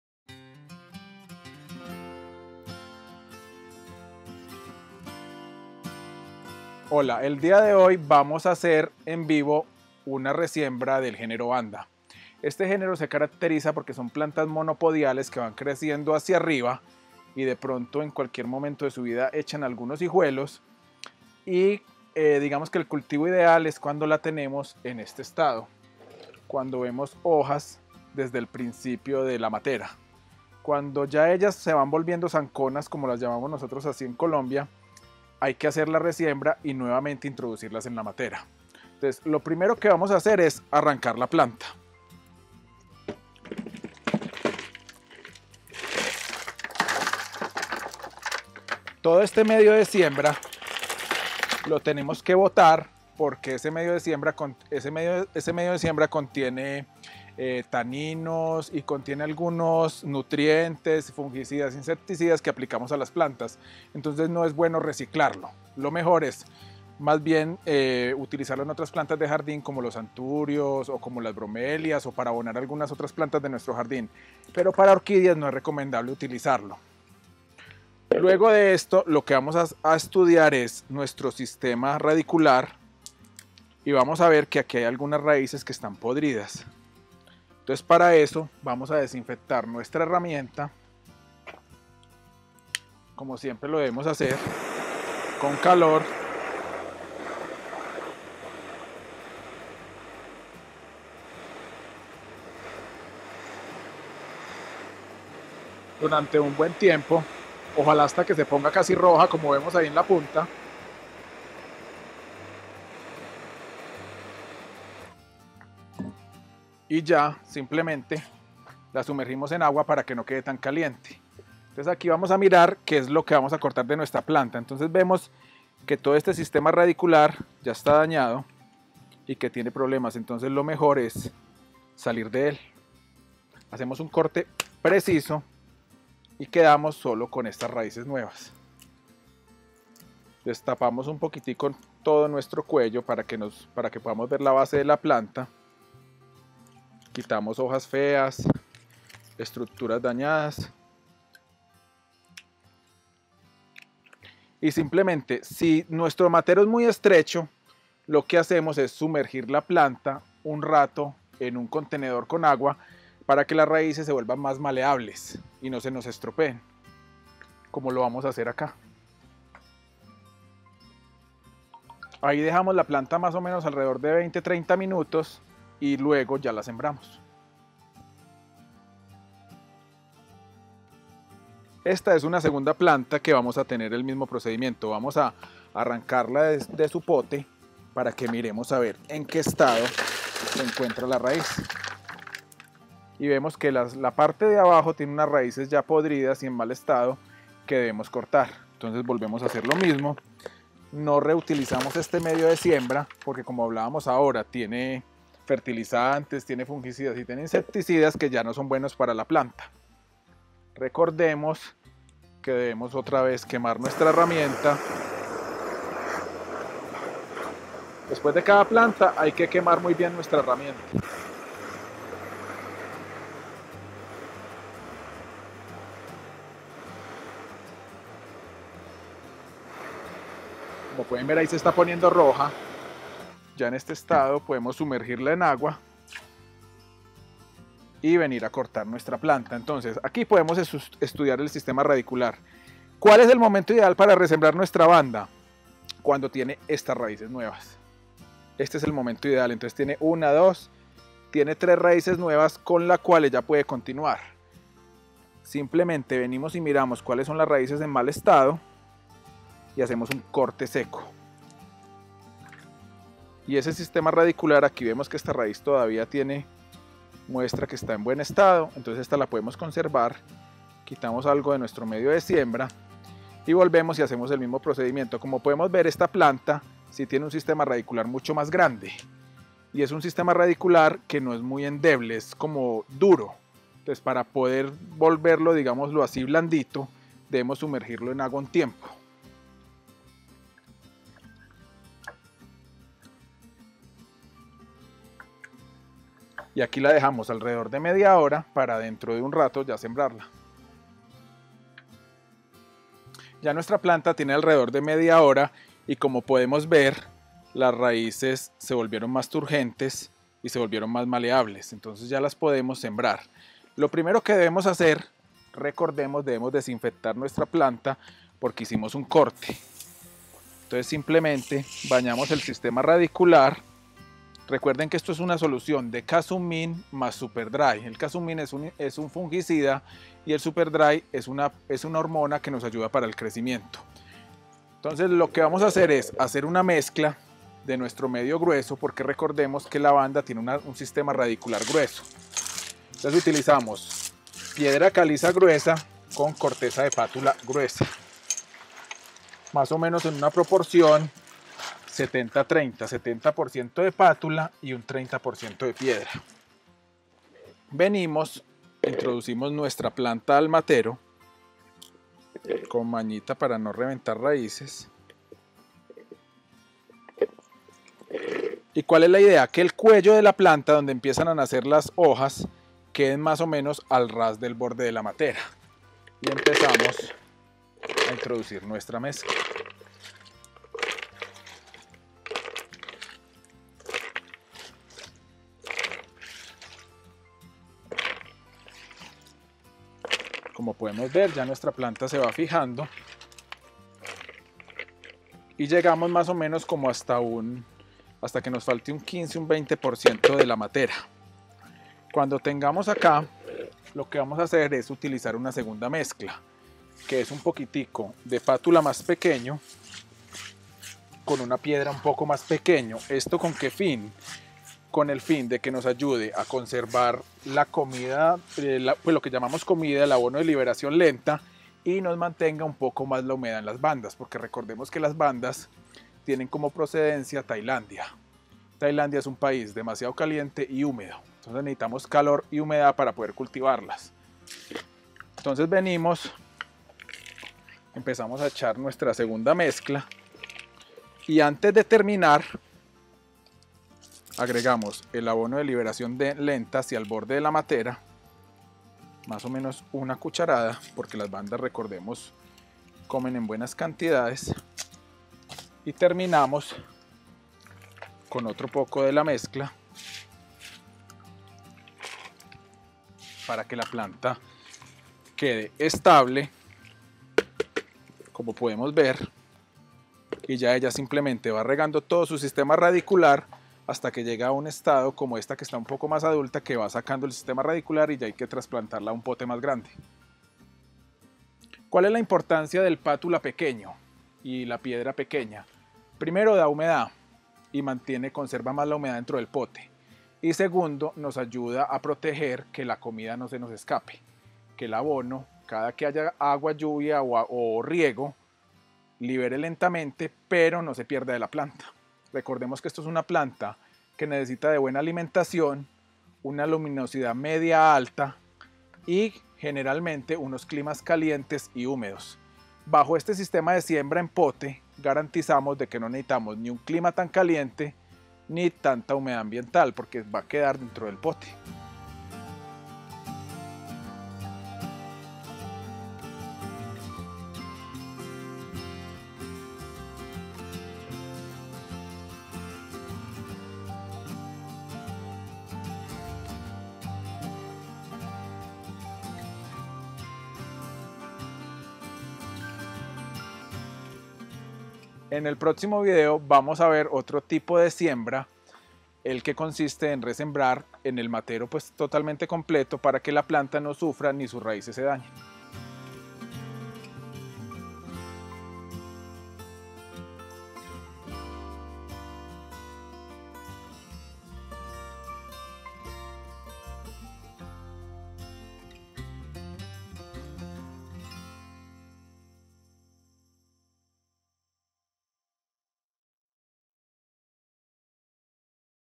Resiembra de Orquídea Vanda - Corte y Reparación de Raíces
En esta ocasión queremos explicarte como realizar la resiembra de un orquídea Vanda cuando esta requiere corte de raíces viejas y mantenimiento general. En nuestro próximo video podrás ver otra técnica de resiembra, quédate conectado y aprende con nosotros.